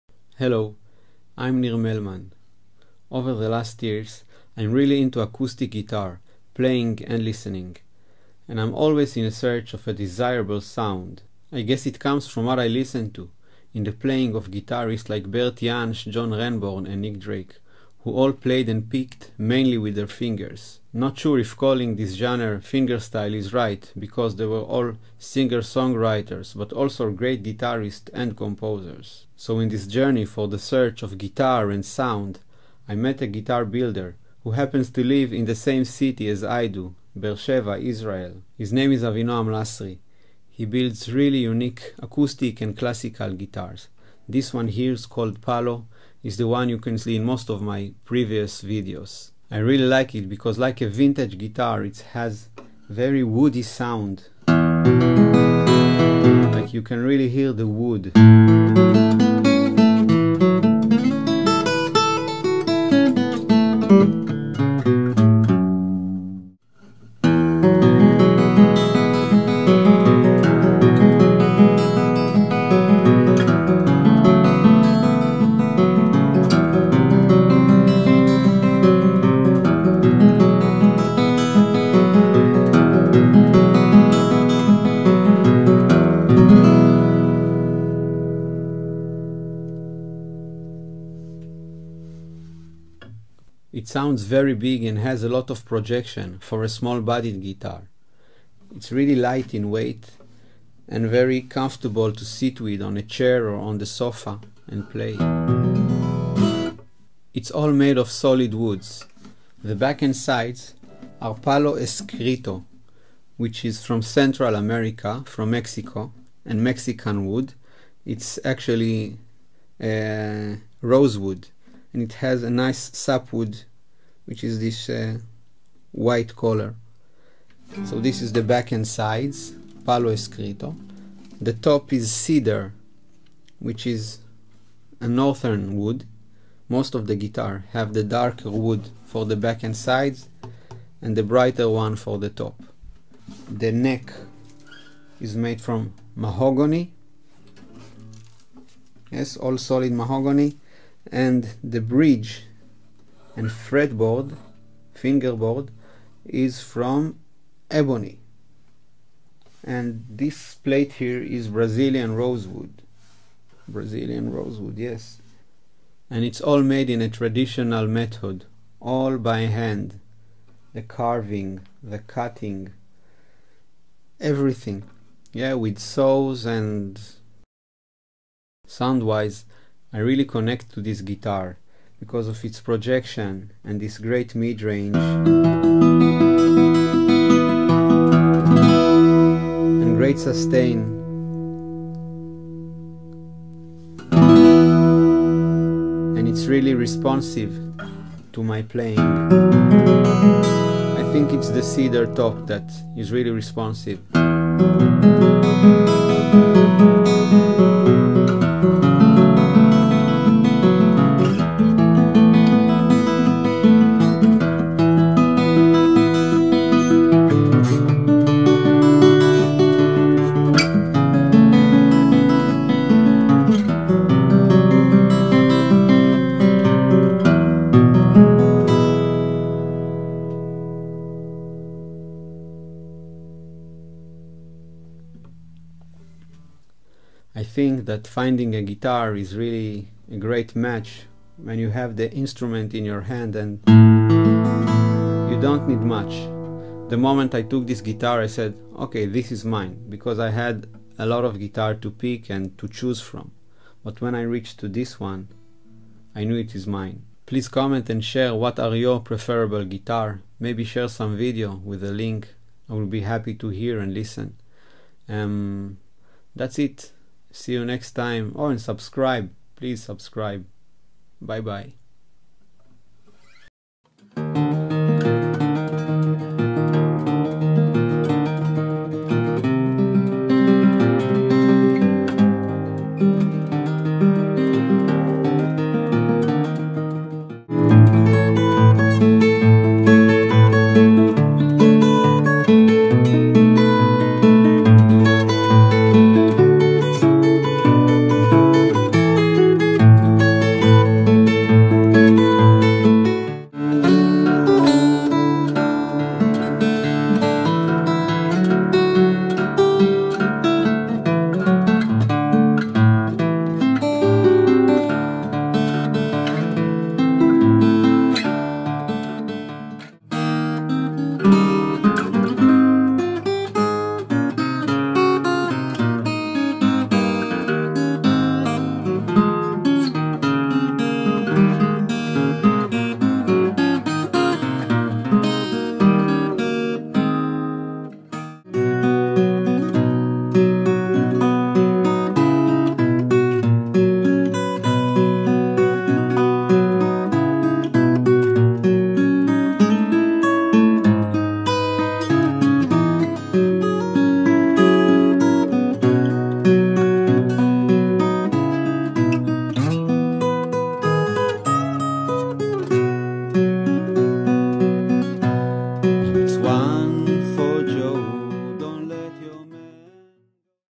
MY SPECIAL HAND BUILT ACOUSTIC GUITAR
I would like to introduce you one of my acoustic guitars, the one I'm playing in many videos lately. It's a very special one, it was hand built in a traditional method by a friend of mine, Avinoam Lasry, he builds very unique acoustic and classic guitars,  this model called Palo Escrito, in name of the wood that it's made of, it's a Rosewood from Mexico. I really enjoy this guitar, the sound and how it feels in my hands, it sends me the sound of guitars played by my favorite artists, like Nick Drake, Bert Jansch, John Renbourn, Jackson C. Frank.